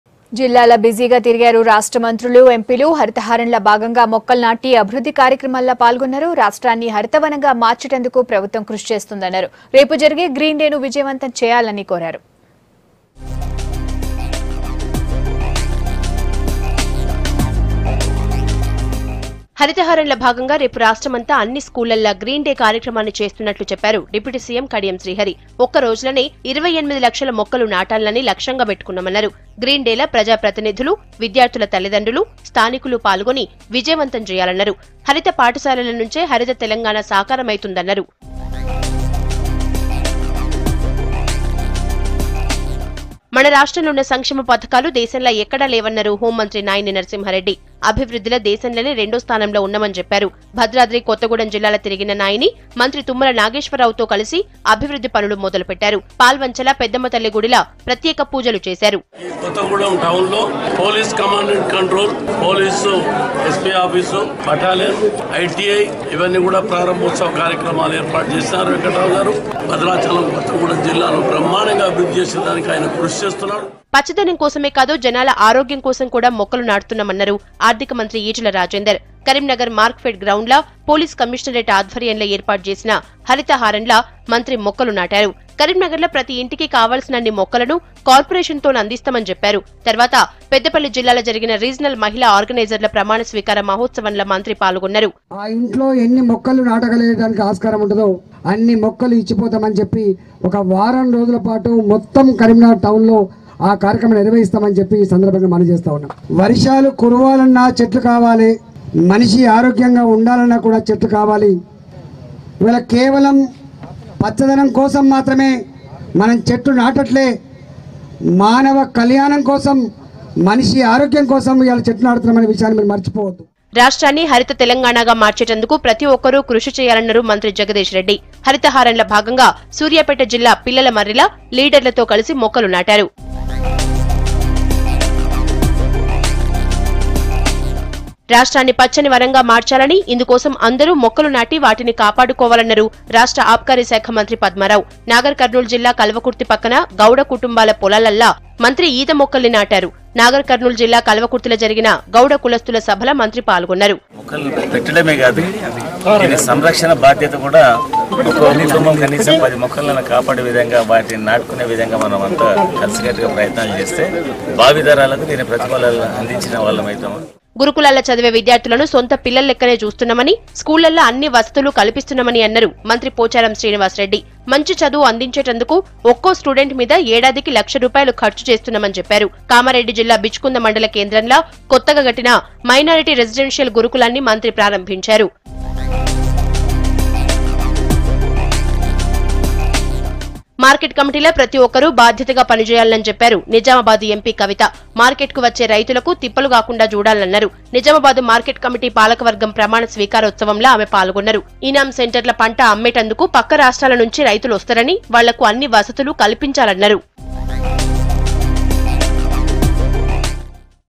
Telangana Ministers Busy In District Tours | Participates In Haritha Haram | V6 News
Telangana Ministers Busy In District Tour | Participates In Haritha Haram Program.